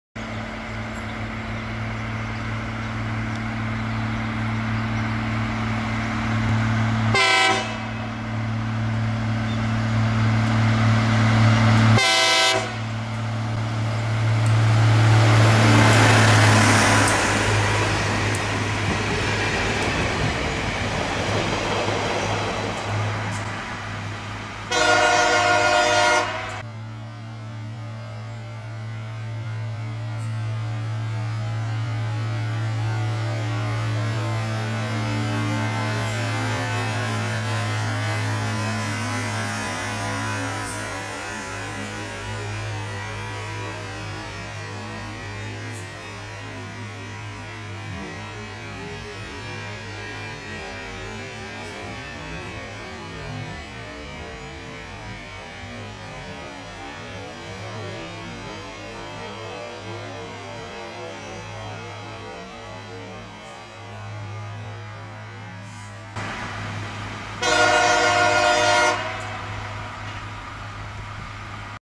What Does This Train Do???
Spotted at Forbes in NSW Australia. Please add a comment if you know what this train is and what it does.